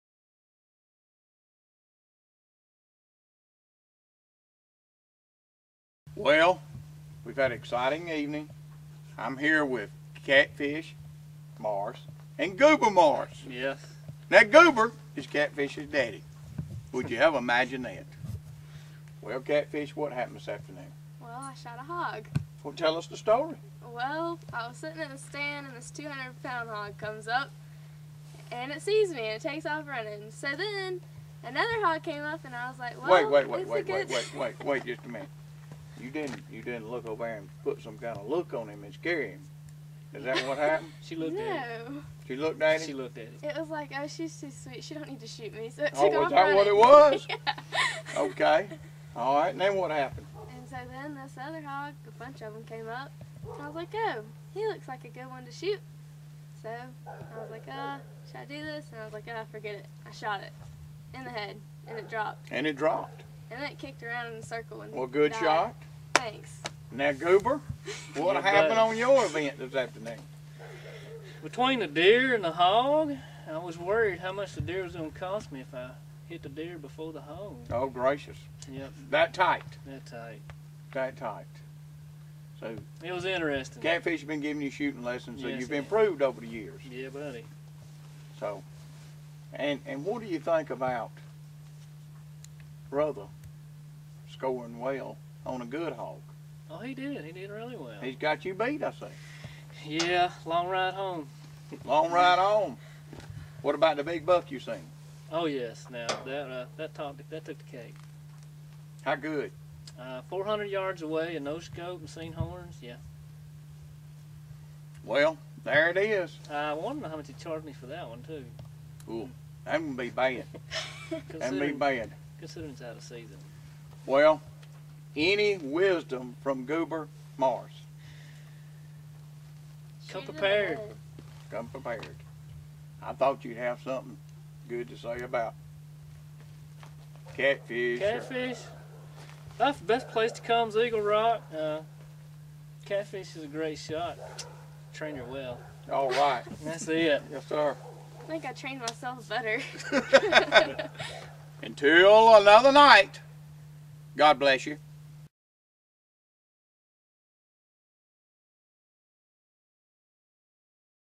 Eagle Rock Plantation -Johnny and Rachel Morris
A successful father and daughter hunt at Eagle Rock Plantation